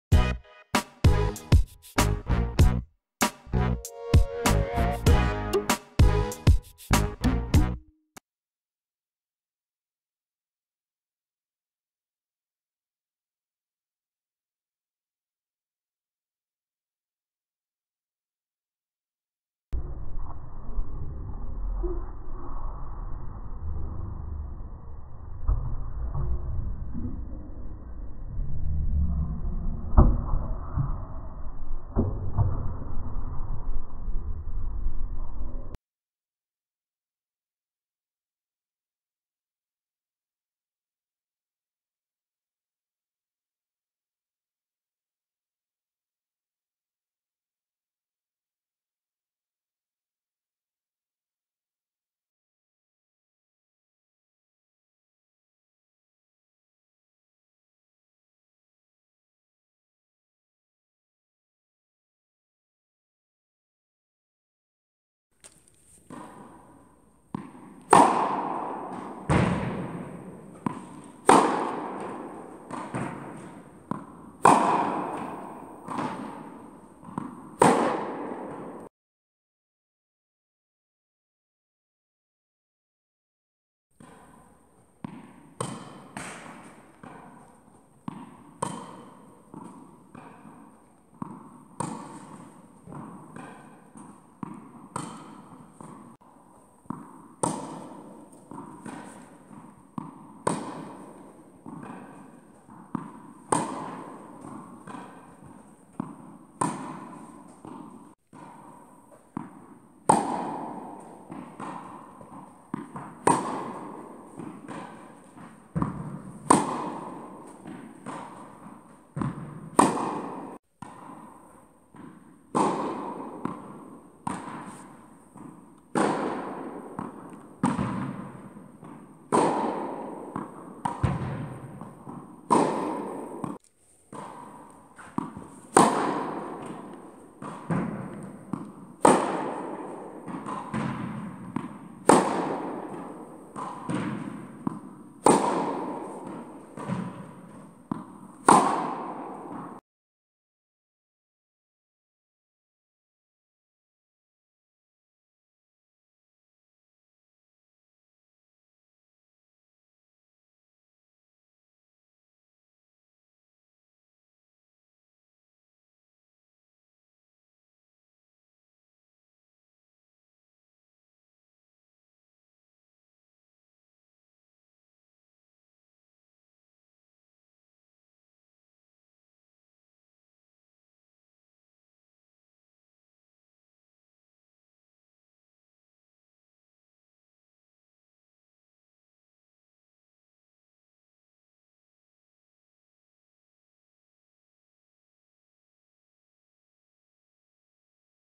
Tennis Forehand Technique - How to easily improve a complicated Forehand Backswing
Hey guys, here is a new video with a drill for the correction of a loopy or complicated backswing on a tennis forehand. All you need is a physioball. Looks kinda weird but it always works. So just check it out! If you want more information about the "RoPe Tennis Education" project please watch our series "What we want" or send an email to
Many happy returns  ;)  Cheers guys